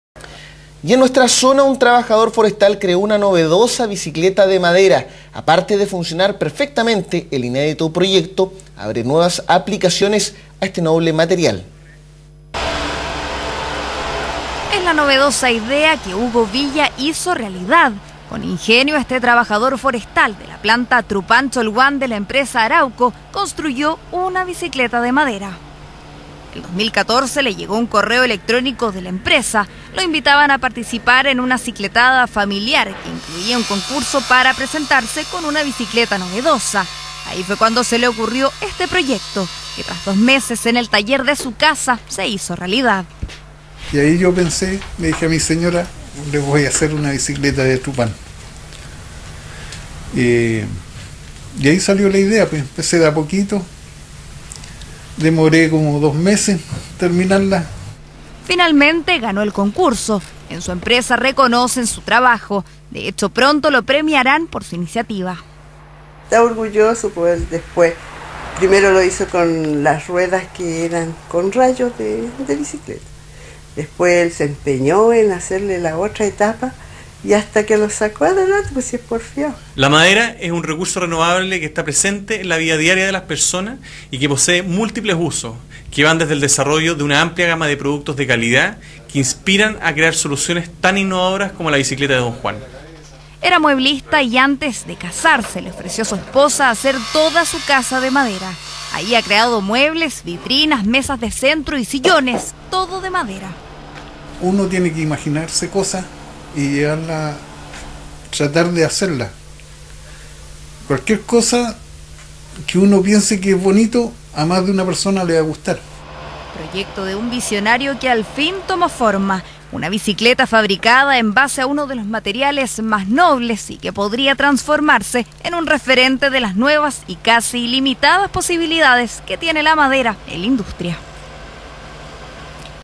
Canal 13 Bicicleta de Madera
Trabajador forestal construye bicicleta de madera
Se trata de Hugo Villa Oñate, edad, trabajador de la Planta Trupán Cholguán de la empresa Arauco, quien construyó una bicicleta de madera.
El año pasado en su trabajo le llegó un mail de la empresa, donde lo invitaban a participar en una cicletada familiar, que incluía un concurso para presentarse con una bicicleta novedosa.
Por lo anterior, a Hugo se le ocurrió una idea que comentó con su señora y compañeros de trabajo… era la de construir una bicicleta de madera.
Luego de dos meses de trabajo en el taller de su casa, logró terminarla y presentarla en la cicletada, donde ganó el concurso.
Antes de llegar a trabajar a la Planta, él era mueblista y fue una de las cosas que le ofreció a su señora para casarse…. Hacer toda su casa de madera, en donde ha creado muebles con madera MDF (Fibro panel de densidad media). Ha construido vitrinas, mesas de centro y sillones.